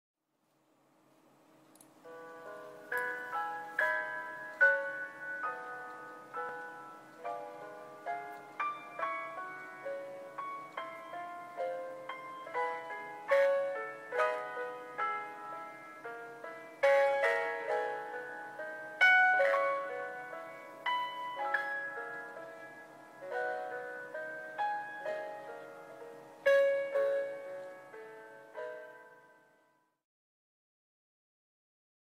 Fipa Maiora 20
Toscana  viareggio fipa maiora 20 1993 maiora 20 barche a motore  400 2 x 1150 hp - mtu 1150 - anno: 1993 - ore moto: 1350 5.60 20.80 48 maiora 20 del 1993 sempre seguita e con refit costanti. tre cabine piu\' cabina marinaio. mobili in radica di betulla  con 2 mtu da 1150 hp .accessori: allarme sentina  ancora  angolo barra  antenna satellitare tv  aria condizionata  autoclave  autogonfiabile  batterie  boiler  bussola  caricabatterie  contamiglia  cucina  cuscineria fly  doccia esterna  doppia timoneria  ecoscandaglio  elica di prua - bowtruster  flap idraulici (nÂ°2)  fornello  forno ...
  idAnnuncio: